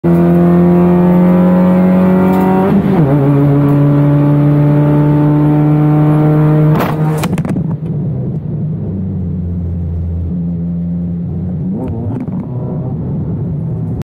Windshield getting SMASHED by bonnet in qualifying | MRF Saloon cup | MMRT
Smashed the windshield at 140 + kph in qualifying.
First lap in Qualifying, no lap set, started right at the back.